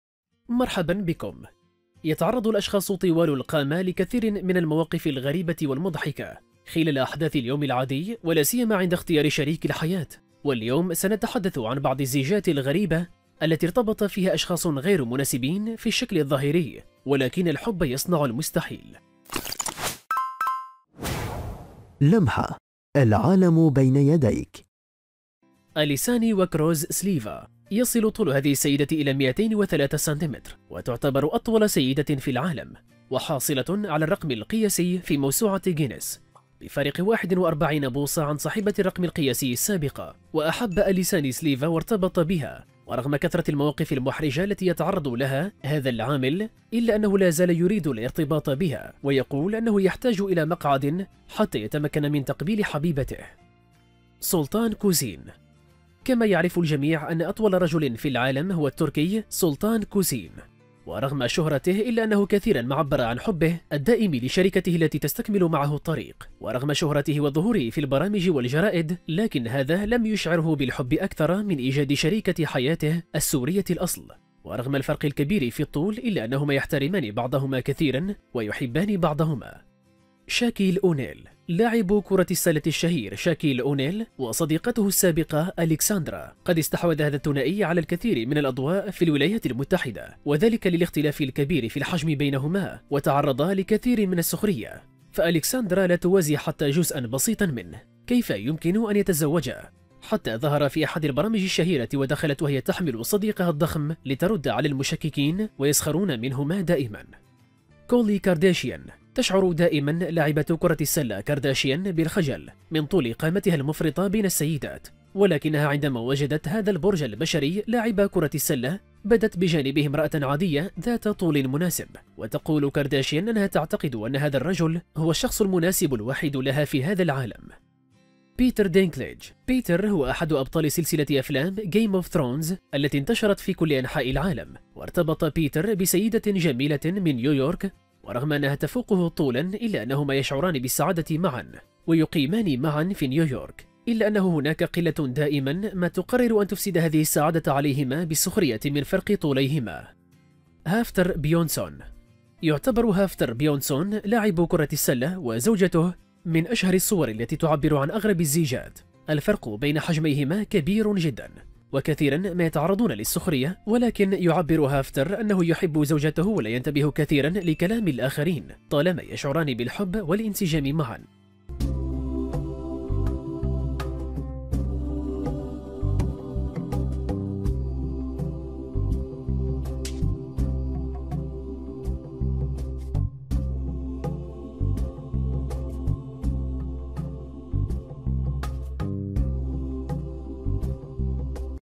العشق الممنوع .. اغرب الأزواج حول العالم !!
يتعرض الاشخاص طوال القامة لكثير من المواقف الغريبة والمضحكة خلال احداث اليوم العادى ولاسيما عند اختيار شريك الحياة واليوم سنتحدث عن بعض الزيجات الغريبة التى ارتبط فيها اشخاص غير مناسبين فى الشكل الظاهري ولكن الحب يصنع المستحيل

( لمحة ) | العالم بين يديك
أشترك الآن وقم بتفعيل زر الجرس ليصلك كل جديد